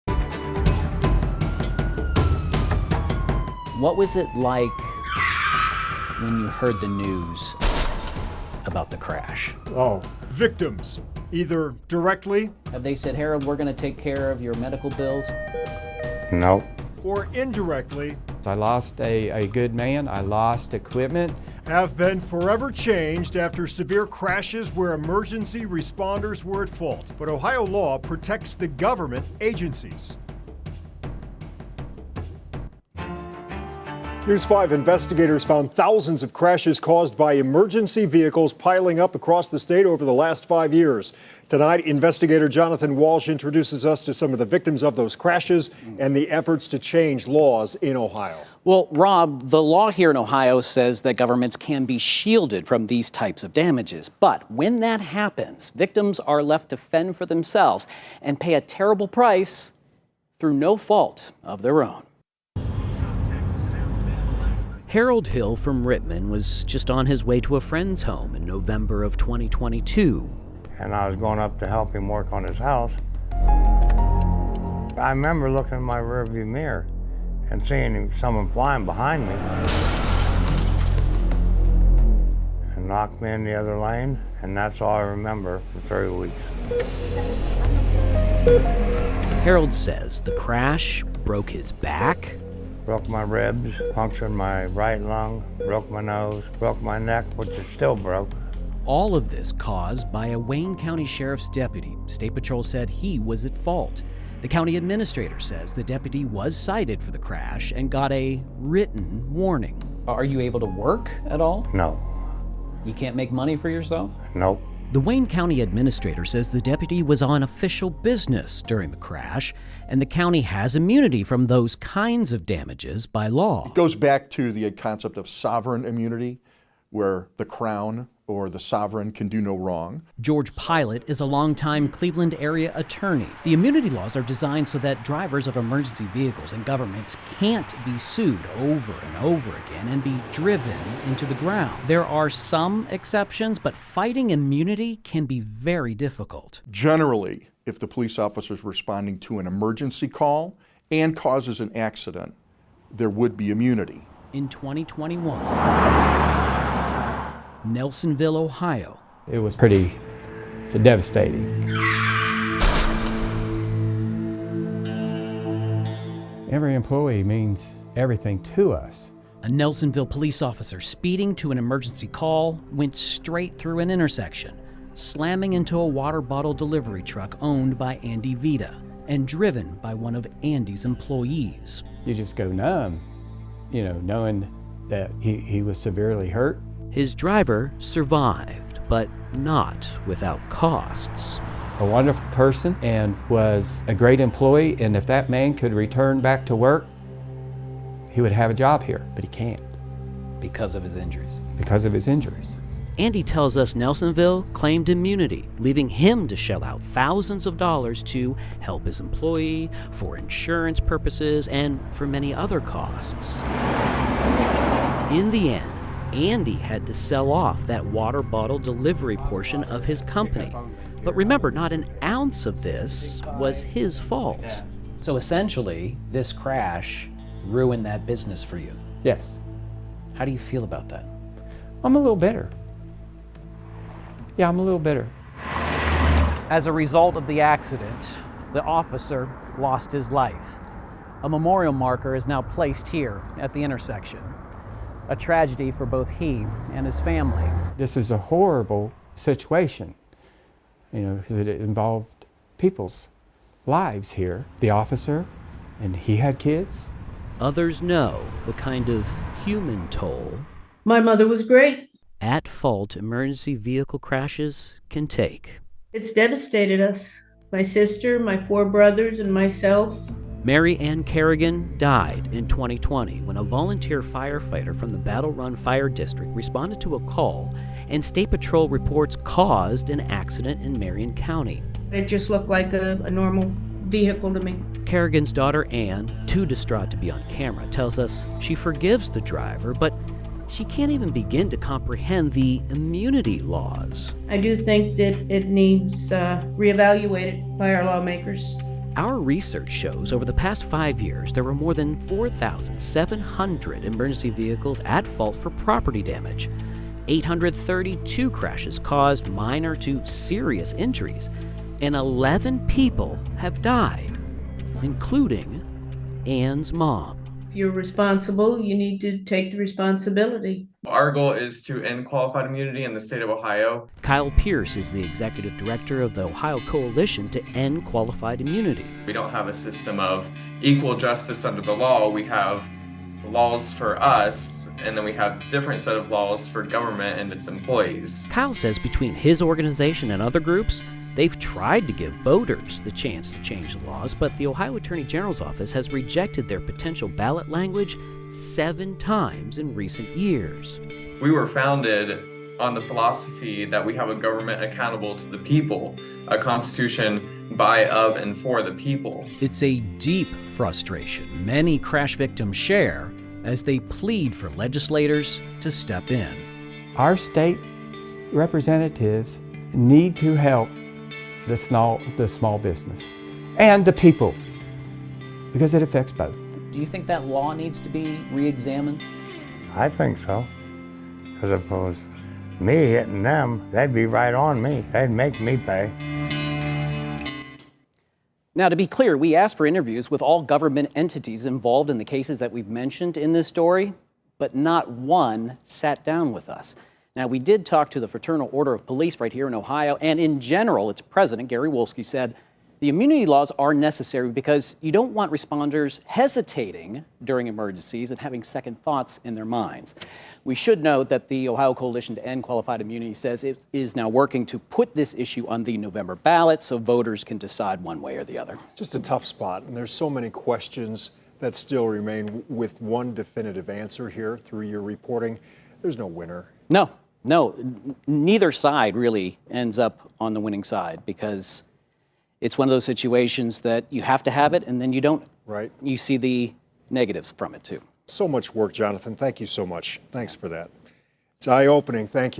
Ohio emergency vehicles at fault for crashes; immunity laws questioned
Victims either directly or indirectly have been forever changed after severe crashes caused by emergency responders, but there are Ohio laws that protect the government agencies.